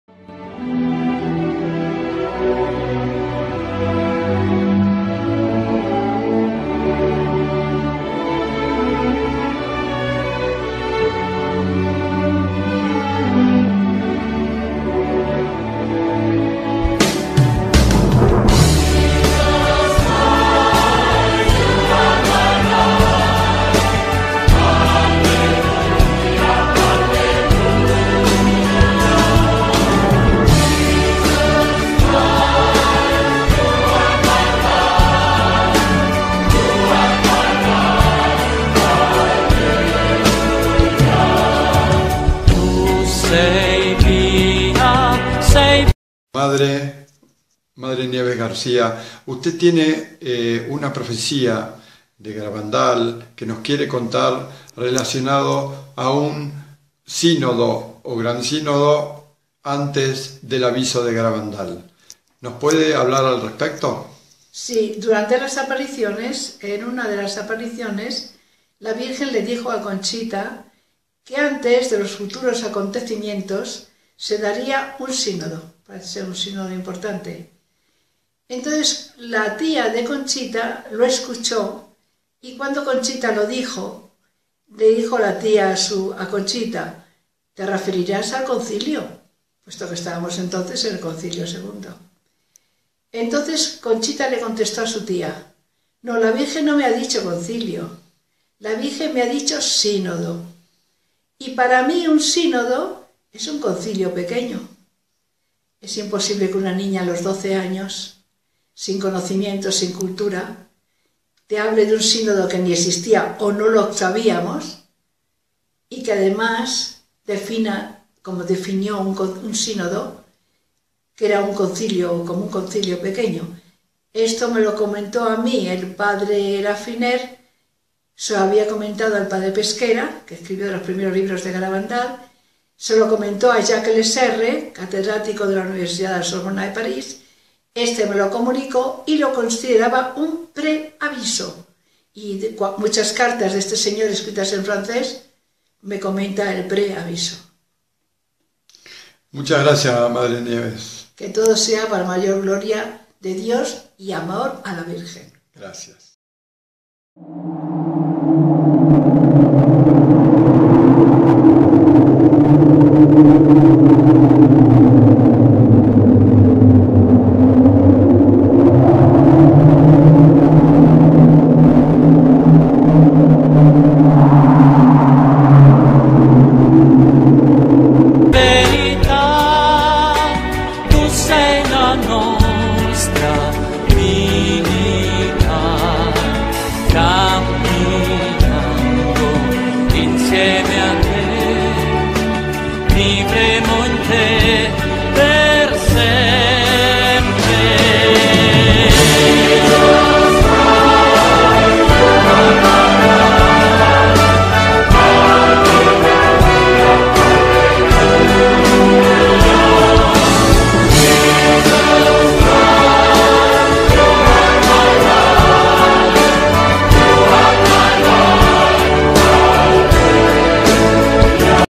Garabandal: Antes de AVISO habrá un PREAVISO (Conchita 1968)
La Virgen María le comunicó a Conchita (vidente de Garabandal) que antes del Aviso sucederá un PreAviso.
La Madre Nieves García (Orden Concepcionista) cuando Conchita fue internada en Burgos en 1966 (un año después de acabar las apariciones) comunicó a la Madre Nieves lo del Preaviso.

Pero..¿QUÉ ES UN SÍNODO?
Un sínodo de los Obispos es una asamblea de Obispos escogidos de las distintas regiones del mundo, que se reúnen en ocasiones determinadas para fomentar la unión estrecha entre el Romano Pontífice y los Obispos,  y ayudar al Papa con sus consejos para la integridad y mejora de la fe y  costumbres y la conservación y fortalecimiento de la disciplina eclesiástica, y estudiar las cuestiones que se refieren a la acción de la Iglesia en el mundo.
Fue creado (el Sínodo) por el Papa Pablo VI el 15 Septiembre de 1965, en respuesta a los deseos de los Padres del Concilio Vaticano II de mantener vivo el buen espíritu nacido de la experiencia conciliar.
El sínodo tiene dos tipos de asambleas: asambleas generales y asambleas especiales. Las Asambleas generales del Sínodo de los Obispos tratan asuntos concernientes a toda la Iglesia; mientras que las Asambleas especiales tratan cuestiones que conciernen directamente a Iglesias y regiones determinadas.